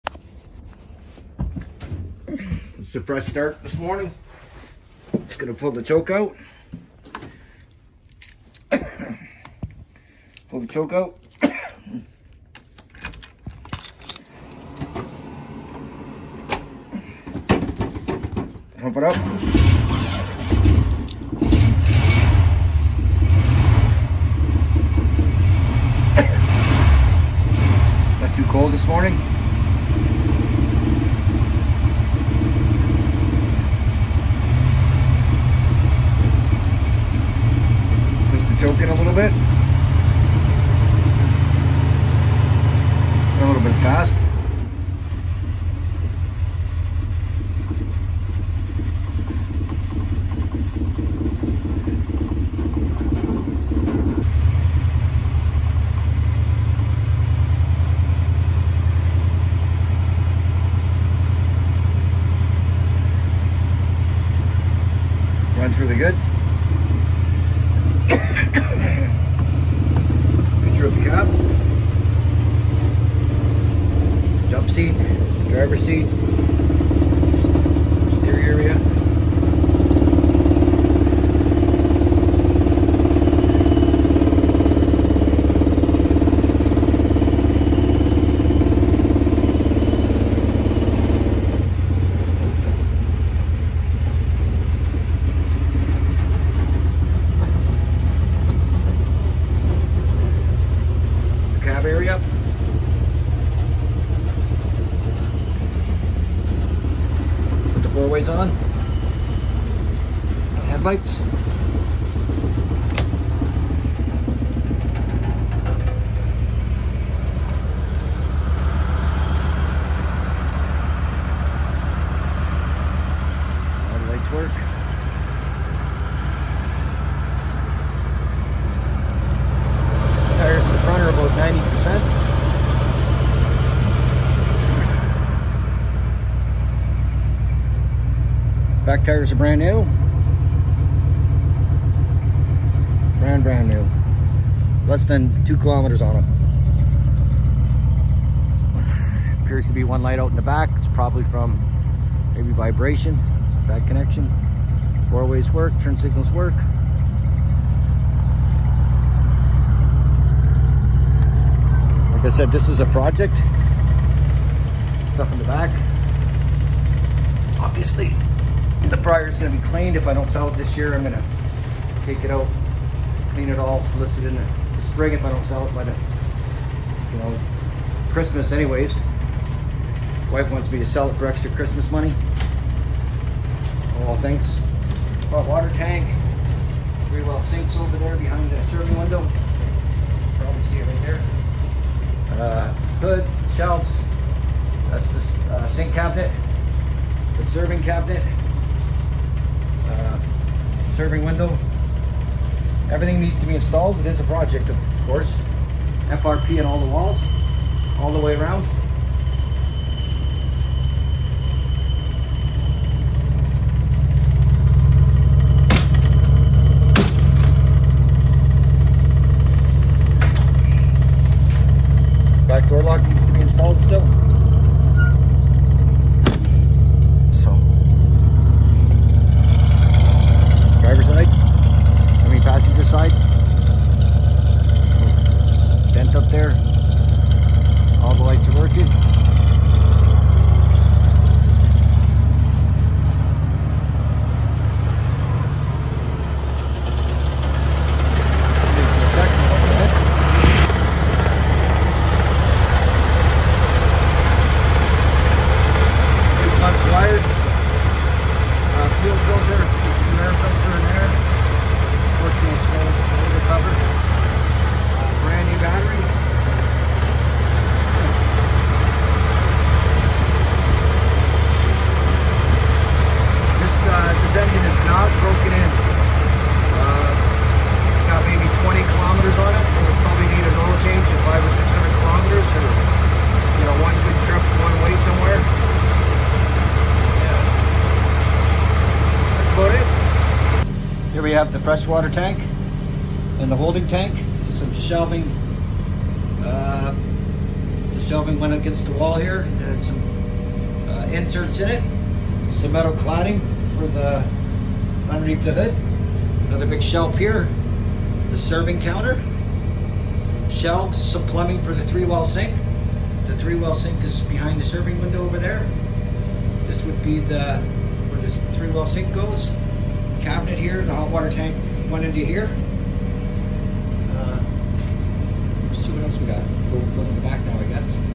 1987 Grumman food truck project
this video is to show it runs good